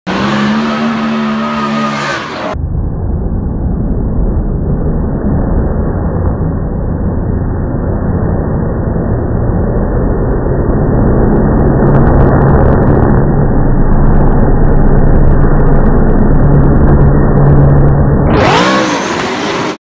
FDLB Friday Practice- Gittin vs Hamilton
2015 Formula Drift Long Beach Friday Practice- Vaughn Gittin Jr vs Nate Hamilton